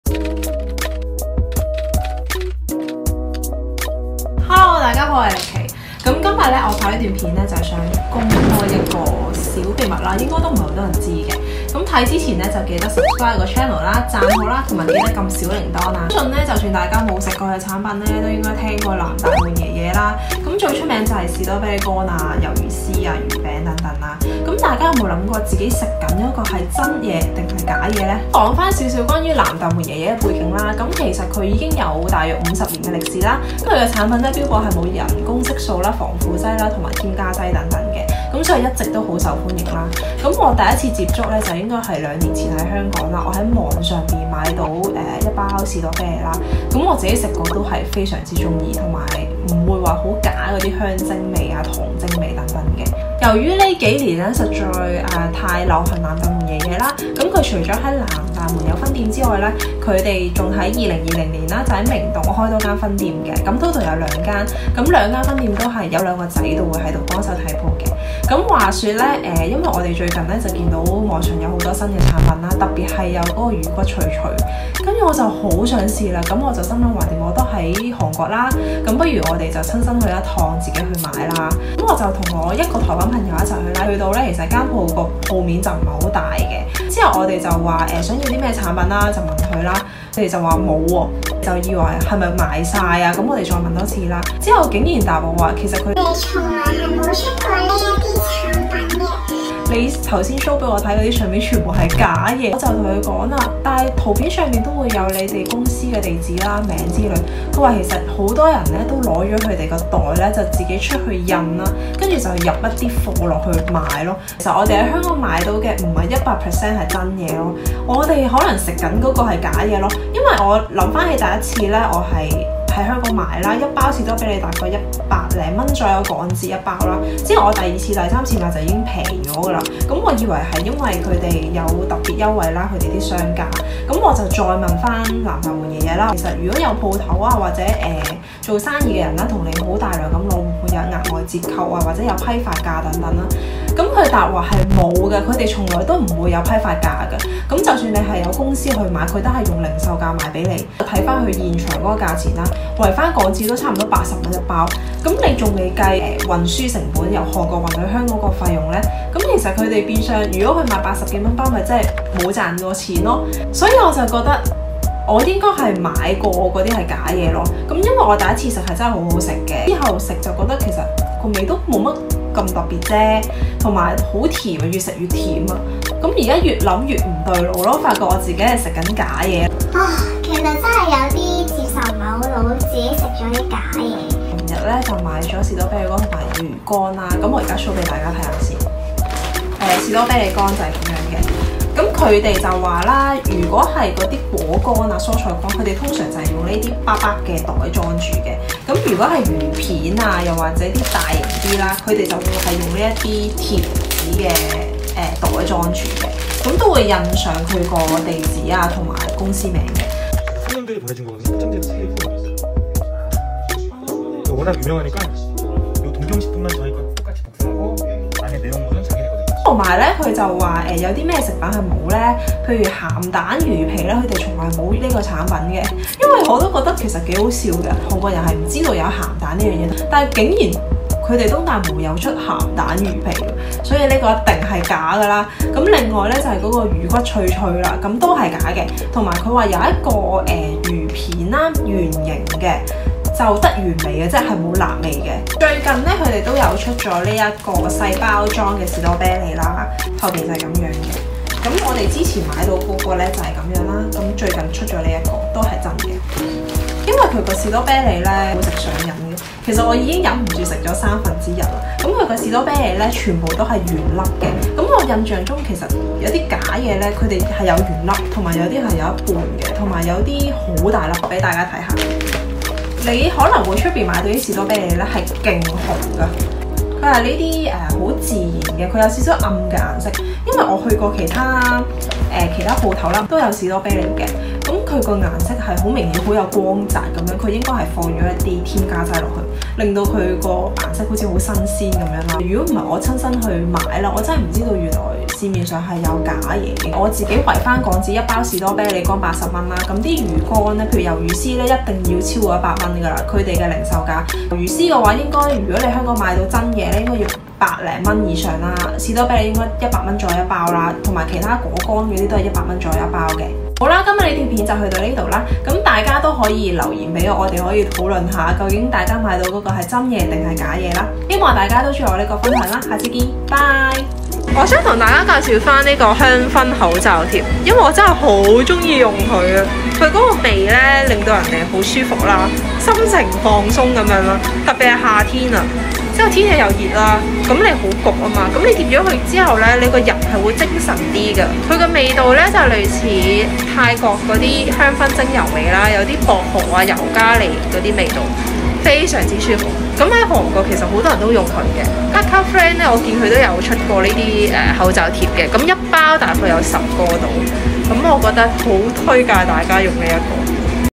［驚人秘密］南大門老爺爺(東京食品)果乾｜你買到的是正品嗎‼️片尾介紹超好用口罩香薰貼
大家有否想過自己買到的不是正品的南大門老爺爺？因為現在市面上都出現很多冒牌貨😱
包裝裏面的食物來歷不明，想起都可怕！
不是親自去一趟也不會知道的真相🥲
吃在肚裏，大家真的要找信譽良好的商店購買哦！

⚠️店家告訴我們冒牌貨差別！
1. 不良商家會在外面複印膠袋！
2. 店家從來沒有批發價，全部以零售價出售！
(大量取貨也只是包韓國本地郵費)
3. 南大門老爺爺沒有出售過鹹蛋魚皮，魚骨粒，辣味雞泡魚乾，榴槤乾，甜豆乾等食品！

當然在南大門除了老爺爺的果乾有名之外，還有其他果乾店選擇！

如果大家有任何問題歡迎留言💬💬

希望大家都喜歡我的分享啦~~
記得”訂閱”我的頻道和”讚好”👍🏻

FOLLOW ME  ✍🏻